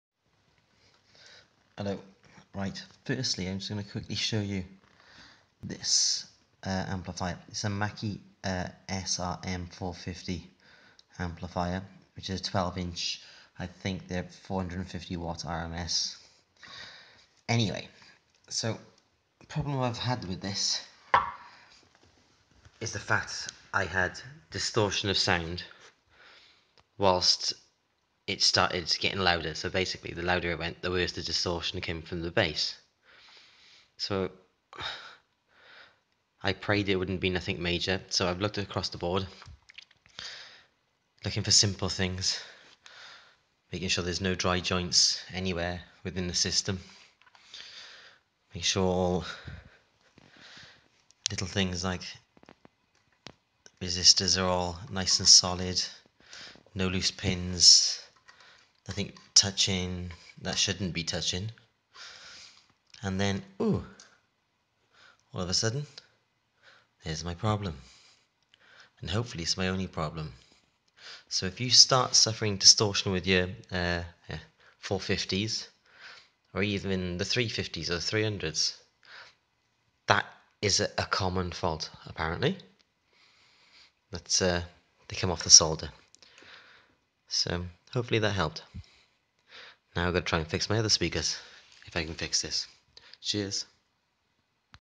Mackie SRM450 Bass Distortion fix.
So around 3 months ago I decided to buy a quality set of second hand speakers so I would not have to repair or renew them,  I decided on a set of Mackie SRM450's 12 Inch Bass drivers with 450w rms inbuilt ( Amplifier ) 3 months after buying them  one has already broken 😠😬. Luckily enough I know a little bit about electrical and electronic engineering so I decided to take the amplifier out and check for loose connections or dry solder joints whilst doing the 1st stage ( visual check ) I was very lucky to find one cement resistor had come off the board 😎 hopefully that is a fix and a nice easy one 😇😇 apparently this is a common fault with Mackie 450 and 350 models.
I hope this was some help and good luck on you're mission.